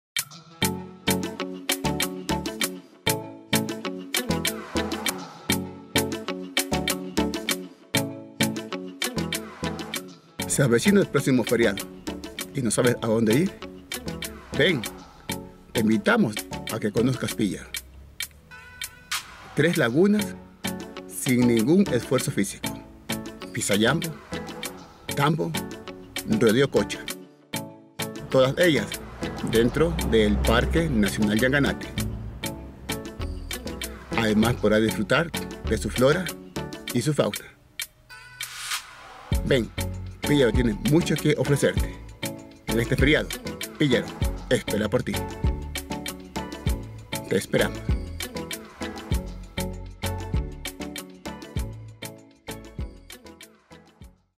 LAGUNAS EN PÍLLARO
En este feriado, te invito a conocer tres lagunas, sin necesidad de hacer esfuerzo físico, las mismas que están dentro del parque Nacional Llanganate.